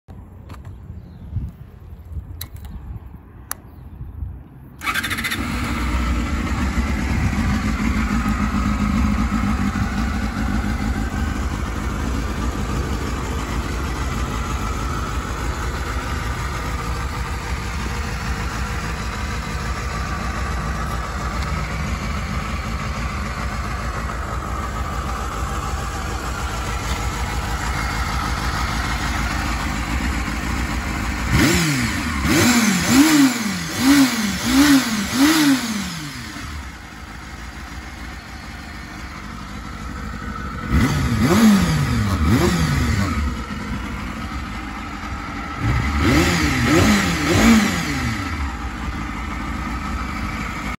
1999 HONDA HORNET CB250F START UP
My 1999 Honda Hornet CB250F Starting Up
Its a 4 Cylinder so it sounds great.

Video Credit To - BitRayte Media - Original Content